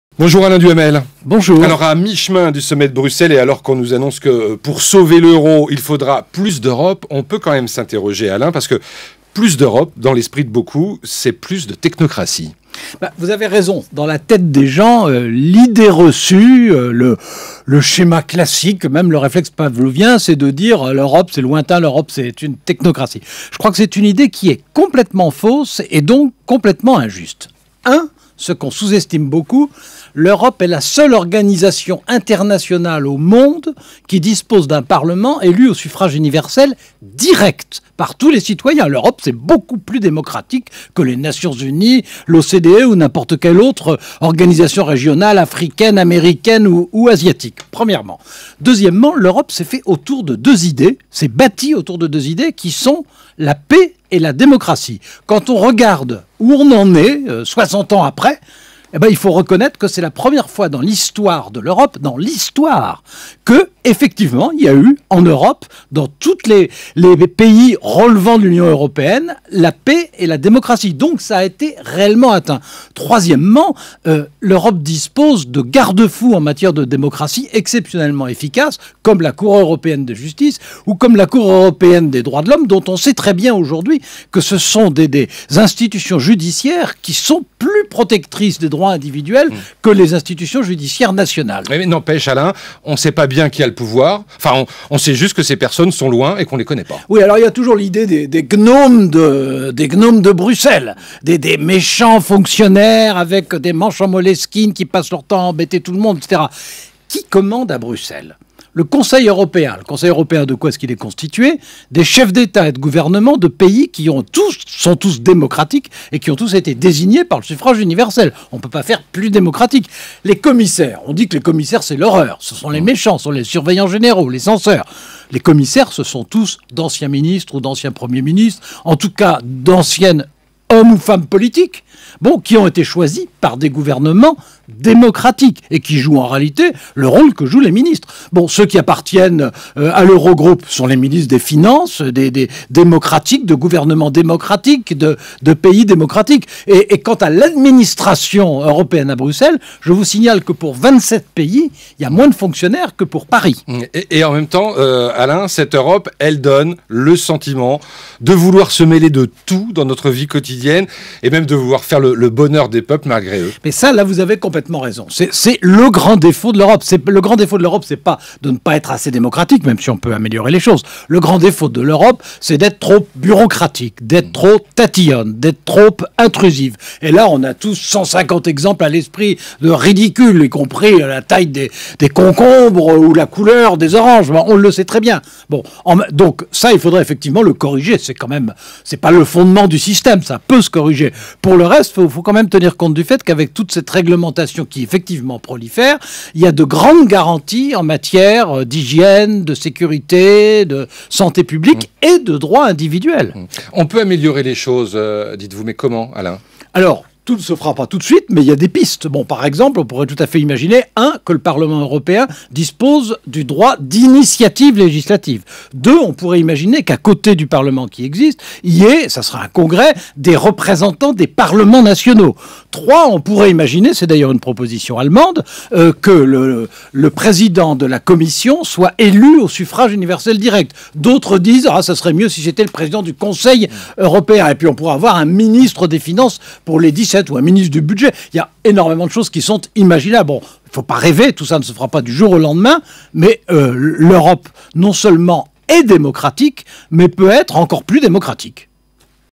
Le défaut de l'Europe, c'est d'être trop intrusive, tatillonne et bureaucratique - RTL - RTL
Le défaut de l'Europe, c'est d'être trop intrusive, tatillonne et bureaucratique